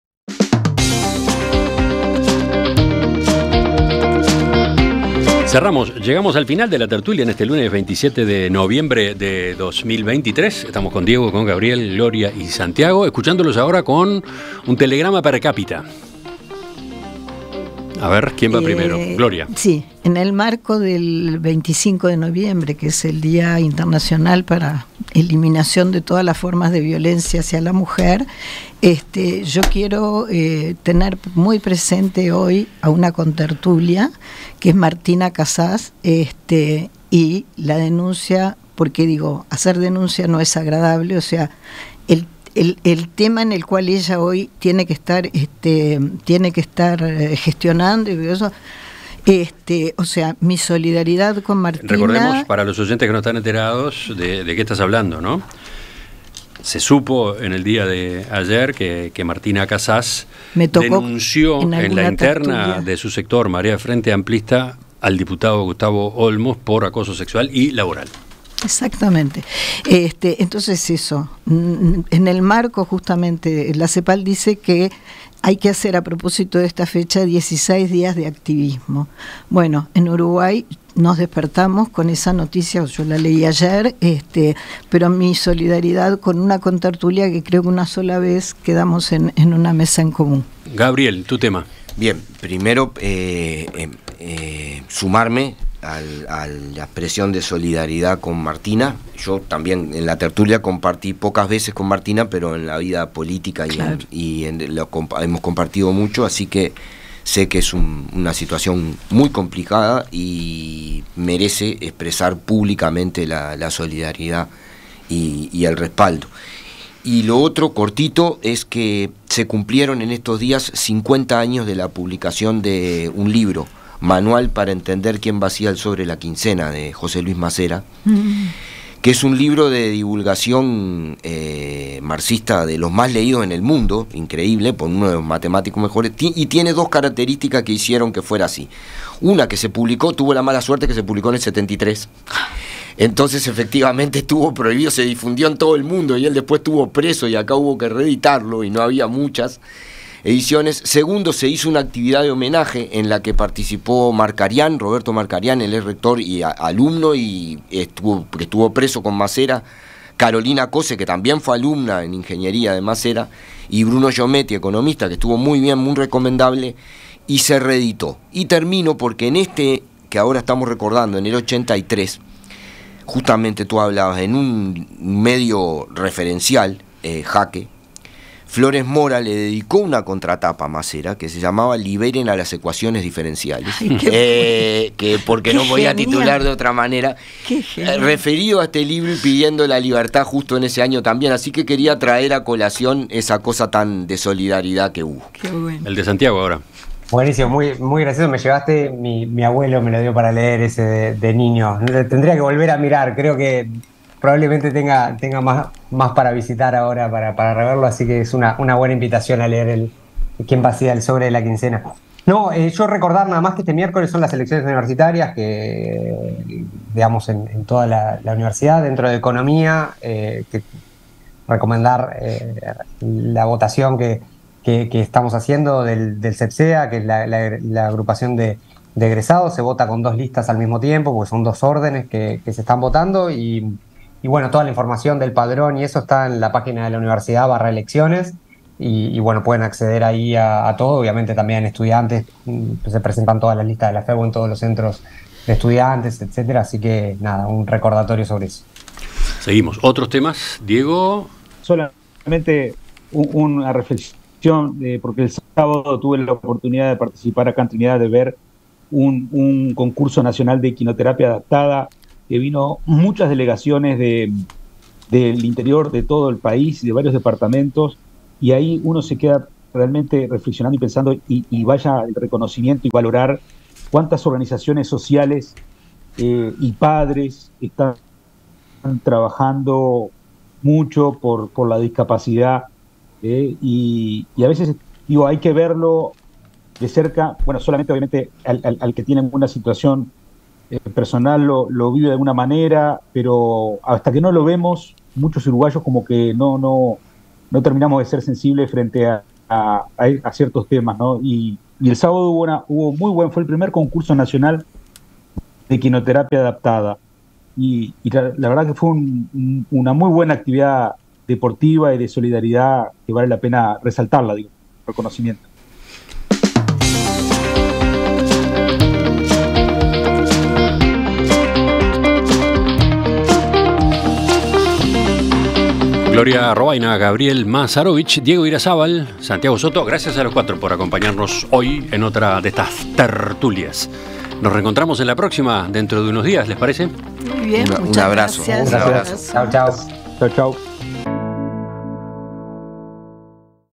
Violencia de género, solidaridad con Martina Casás... y otros telegramas
La Tertulia de los Lunes con Diego Irazábal, Gabriel Mazzarovich, Gloria Robaina y Santiago Soto.